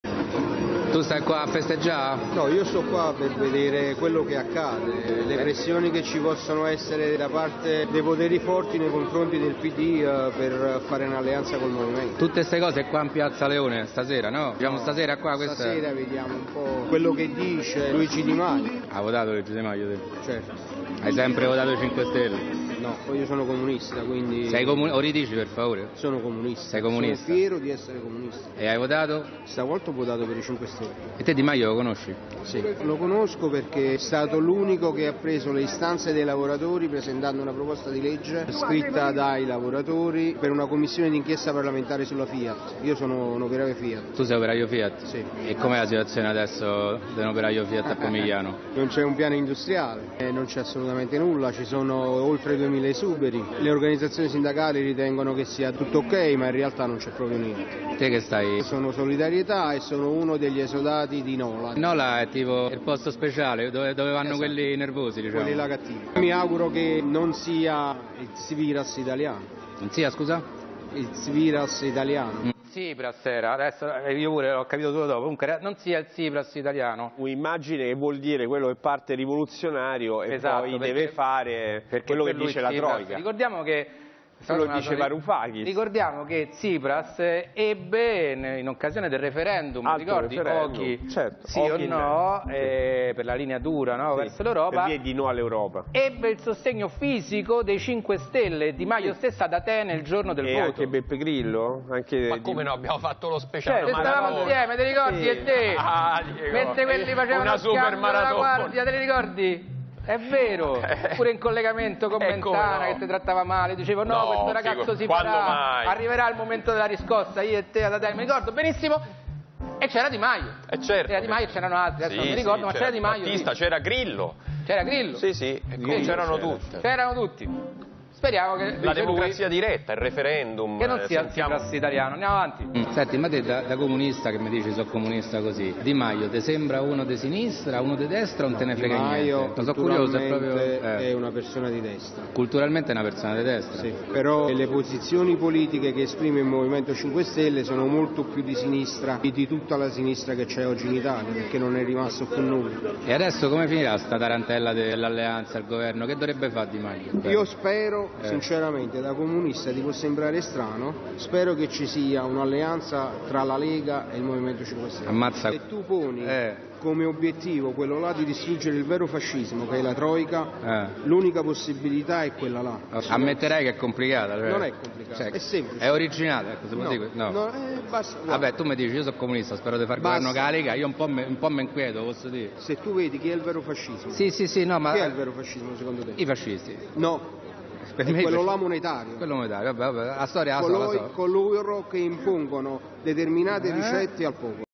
Operaio di Pomigliano vota Cinquestelle: vero fascismo è la Troika
Il comico Diego Bianchi, in arte Zoro, è andato alla festa di Pomigliano del Movimento Cinque Stelle, dove il candidato premier Luigi Di Maio ha festeggiato il successo elettorale del 4 marzo 2018.

Tra i tanti elettori intervistati da Zoro molti sono risultati gli ex comunisti che hanno votato il Movimento Cinque Stelle invece della sinistra storica italiana. Uno di questi, operaio di Pomigliano, auspicava un governo Lega-M5S contro l'Europa: "è quello il vero fascismo, non la Lega" ha spiegato a un interdetto Zoro.

Il popolo italiano è decisamente più avanti dei suoi intellettuali.

Tutti i diritti appartengono a La7